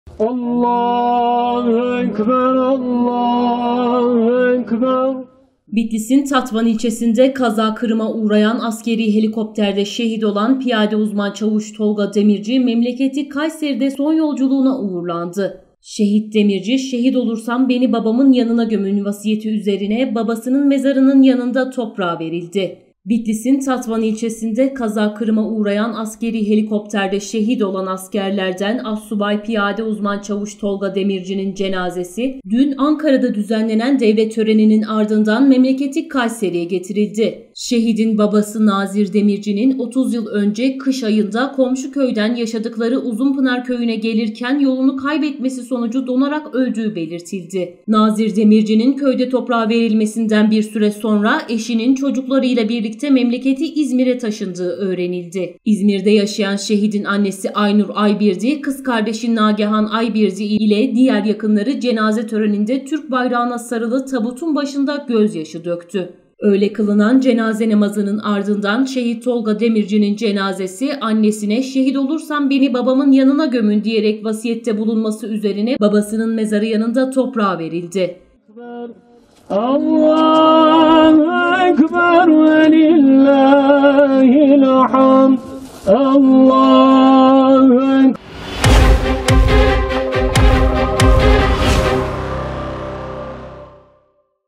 Helikopter kazasında şehit olan Tolga Demirci, Kayseri'de toprağa verildi
BİTLİS'in Tatvan ilçesinde kaza kırıma uğrayan askeri helikopterde şehit olan Piyade Uzman Çavuş Tolga Demirci (33), memleketi Kayseri'de son yolculuğuna uğurlandı. Şehit Demirci, "Şehit olursam beni babamın yanına gömün" vasiyeti üzerine babasının mezarının yanında toprağa verildi.

Bitlis'in Tatvan ilçesinde kaza kırıma uğrayan askeri helikopterde şehit olan askerlerden Astsubay Piyade Uzman Çavuş Tolga Demirci'nin cenazesi, dün Ankara'da düzenlenen devlet töreninin ardından memleketi Kayseri'ye getirildi. Şehit Demirci için, Pınarbaşı ilçesi Uzunpınar köyü mezarlığında cenaze töreni düzenlendi. Törene şehidin ailesinin yanı sıra Vali Şehmus Günaydın, AK Parti Genel Başkan Yardımcısı Mehmet Özhaseki, AK Parti Kayseri Milletvekilleri İsmail Emrah Karayel ve Hülya Nergis Atçı, İYİ Parti Kayseri Milletvekili Dursun Ataş, CHP Kayseri Milletvekili Çetin Arık, Kayseri Büyükşehir Belediye Başkanı Memduh Büyükkılıç, ilçe belediye başkanları, siyasi partilerin temsilcileri ve vatandaşlar katıldı.

VASİYETİ YERİNE GETİRİLDİ

Şehidin babası Nazir Demirci'nin, 30 yıl önce kış ayında komşu köyden yaşadıkları Uzunpınar köyüne yürüyerek gelirken yolunu kaybetmesi sonucu donarak öldüğü belirtildi. Nazir Demirci'nin köyde toprağa verilmesinden bir süre sonra eşinin, çocuklarıyla birlikte memleketi İzmir'e taşındığı öğrenildi. İzmir'de yaşayan şehidin annesi Aynur Aybirdi, kız kardeşi Nagehan Aybirdi ile diğer yakınları, cenaze töreninde Türk bayrağına sarılı tabutun başından gözyaşı döktü. Öğle kılınan cenaze namazının ardından şehit Tolga Demirci'nin cenazesi, annesine, "Şehit olursam beni babamın yanına gömün" diyerek vasiyette bulunması üzerine, babasının mezarının yanında toprağa verildi.